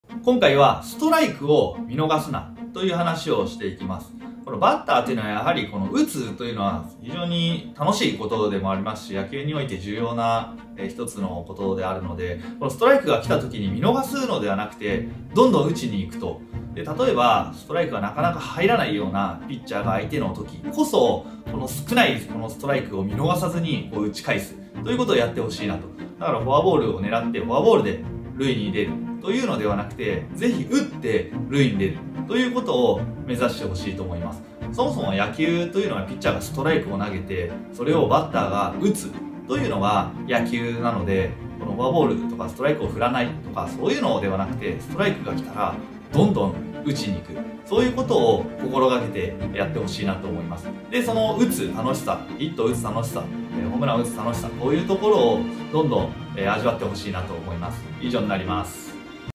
【荻No!チャンネル #112】「ストライクを見逃すな！！」元ロッテのプロ野球投手「ミスターゼロ」こと荻野忠寛が詳しく解説！
野球でメジャーやプロになりたいなら「やってはいけない練習」がある！元ロッテのクローザー「Mr.ゼロ」こと荻野忠寛が本気で教えます！野球少年＆少女はもちろん、少年野球パパ＆ママにもぜひ知ってほしい！